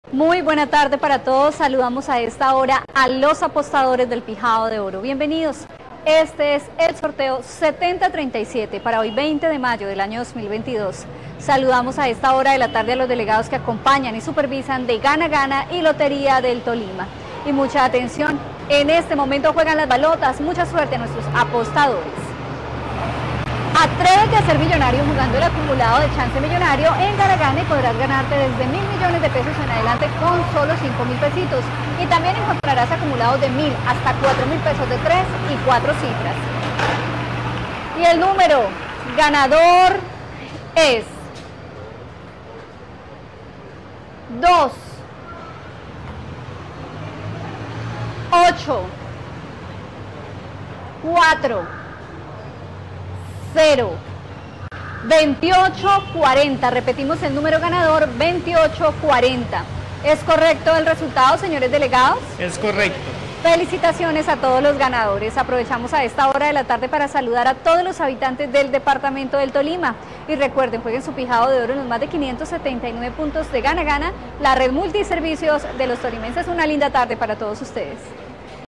PIJAO DE ORO Resultado del VIERNES 20 DE MAYO del 2022 ✅🥇🔥💰
Hola buenas tardes, te presentamos los resultados de las loterías y chances en Colombia, vamos con el Resultados del Chance Pijao de Oro del VIERNES 20 DE MAYO de 2022.